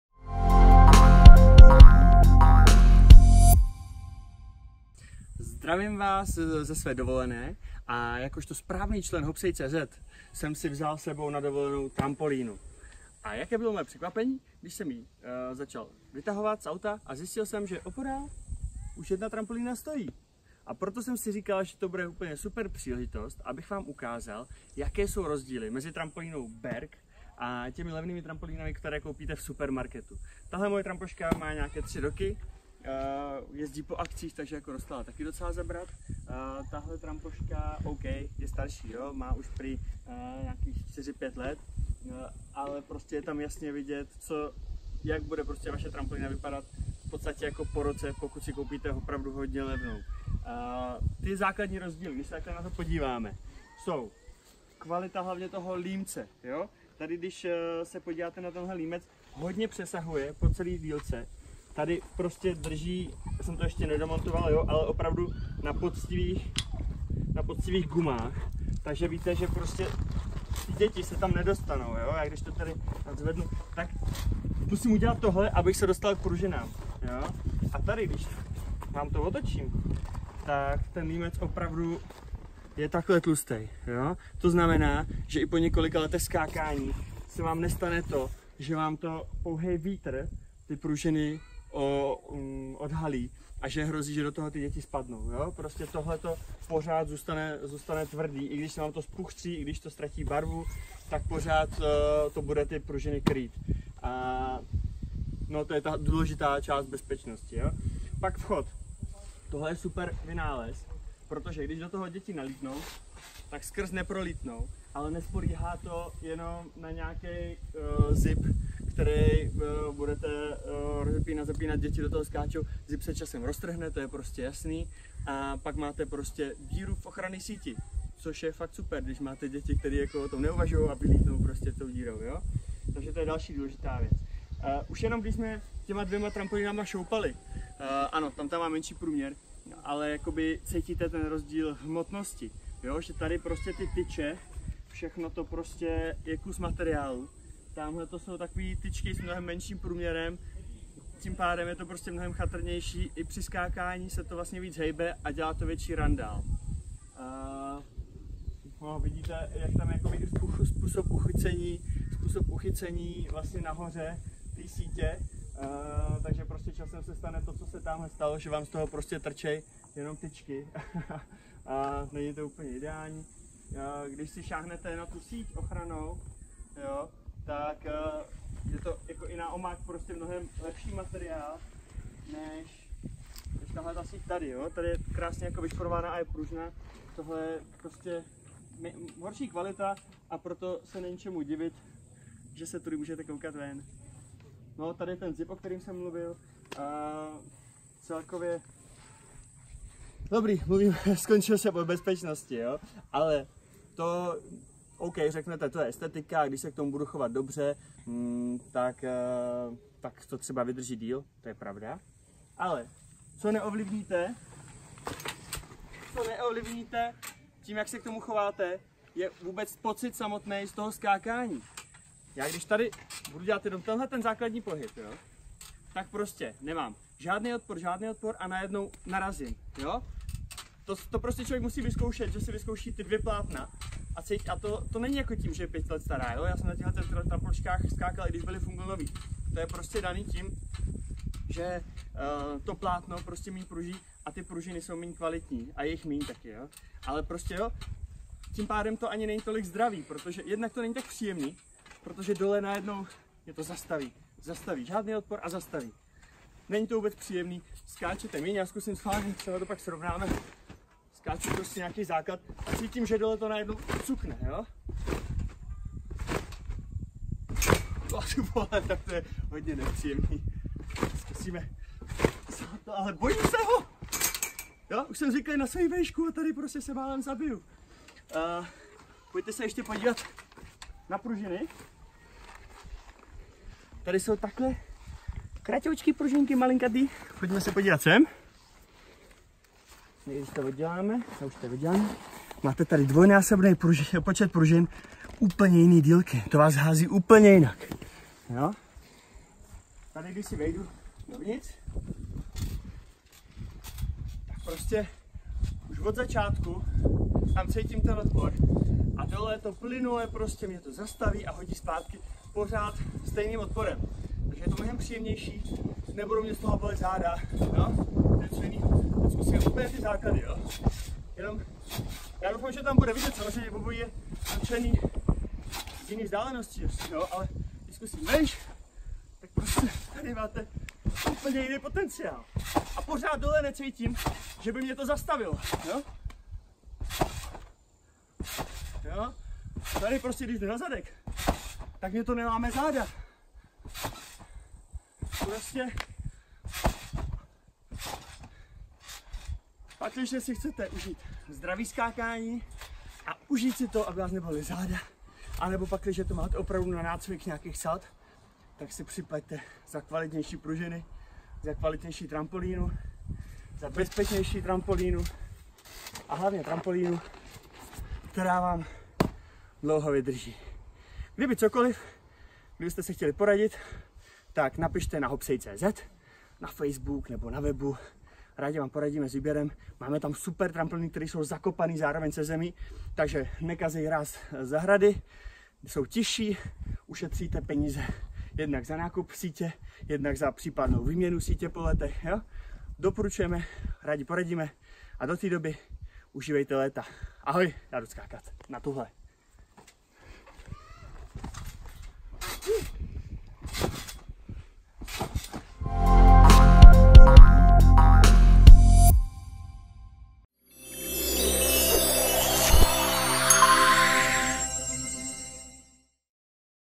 Srovnání trampolín
Bude levná trampolína stačit? Kdy investovat do dražší?  To jsou otázky, které si při nákupu klade každý. V tomto videu si obě varianty srovnáme.